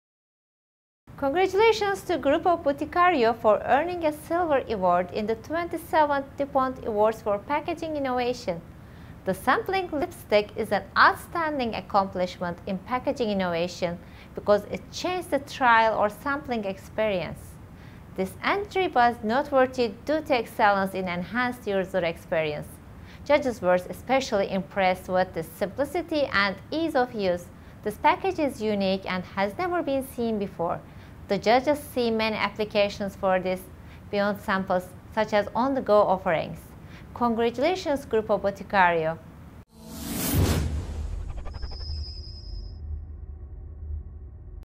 Single-Use Lipstick - 2015 Silver Award Winner
Sampling Lipstick Puts Color on Lips and Confidence in Minds

Grupo Boticario; Ycar Artes Gráficas – Brazil

Single-use lipstick sampling completely changes the lipstick sampling experience in Brazil! Makeup testing can be an unsanitary experience for consumers. The shared sampling pots in stores may become a breeding ground for bacteria and germs that can cause minor to serious skin problems or infections. To solve this issue, Grupo Boticario developed a new way to test lipstick that provides a simple, engaging, and sanitary experience for customers.

The sampling lipstick is a single dose of lipstick “printed” on coated cardboard through a process similar to graphic printing. To test, the consumer simply removes the plastic film, folds the cardboard in half and applies the lipstick directly to her lips, without needing to use her fingers to spread the lipstick on. Each sampling lipstick is used once and then disposed. This simple, easy-to-use sampling solution offers consumers in Brazil a realistic preview of the lipstick color and texture and gives them confidence to purchase the product.

Honored for Excellence in Enhanced User Experience